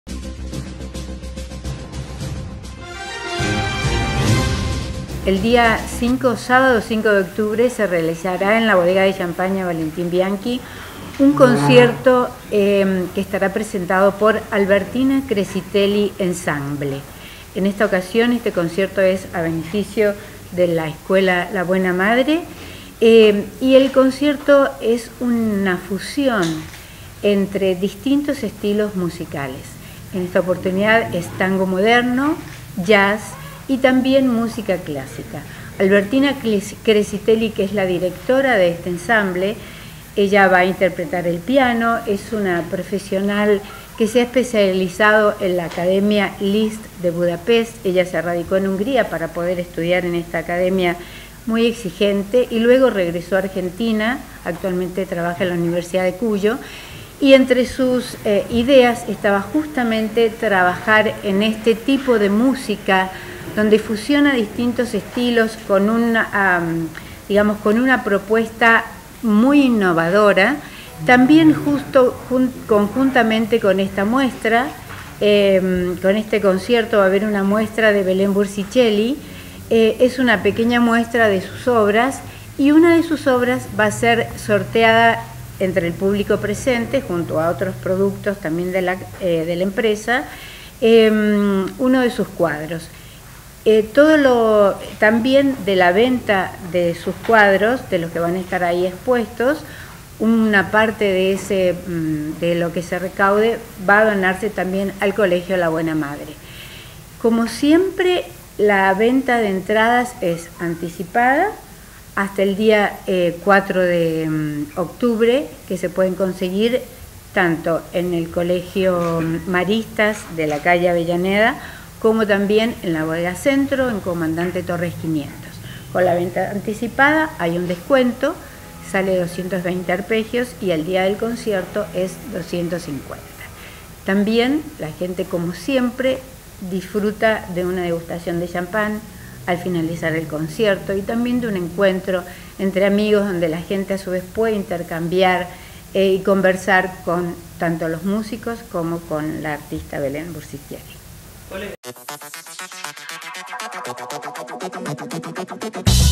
CONCIERTO CON MADERA
Valentina Crescitelli se presentará el próximo sábado 5 en un concierto especial en la Bodega "Valentín Bianchi". El concierto, es a beneficio del colegio "La Buena Madre".